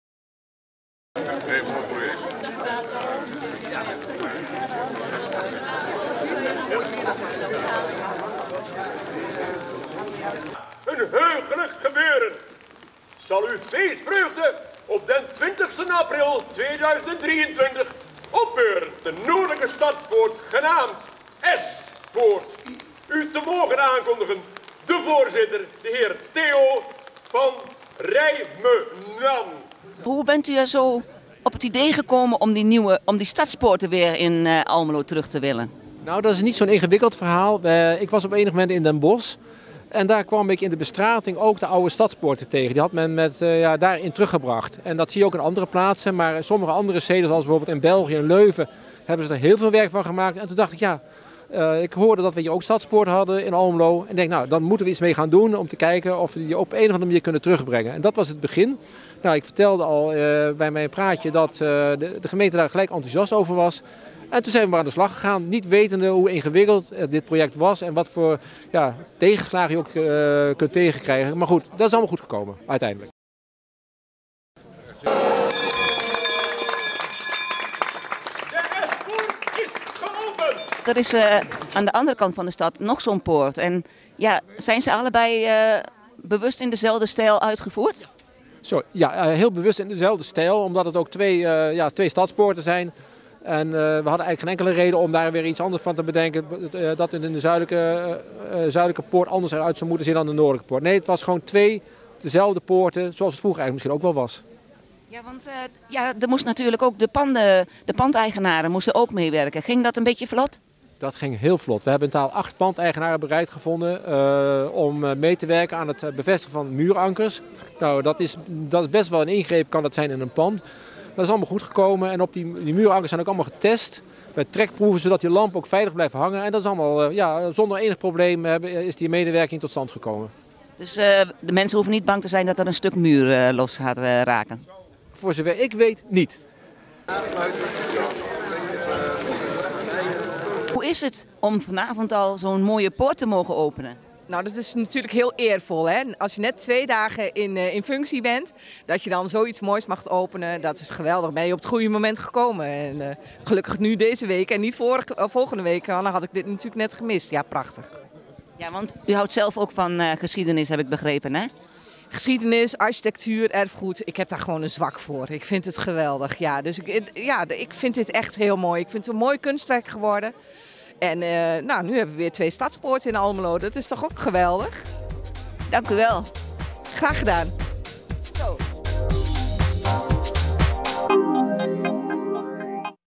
Opening Eschpoort Almelo
Ondanks de regen, was er toch een flinke belangstelling voor het openen van de Eschpoort in de Grotestraat Noord ter hoogte van de Götte en de Hagengracht. Het was Theo van Rijmenam die samen met wethouder Margreet van Overmeen de nieuwe Stadspoort in het licht zette.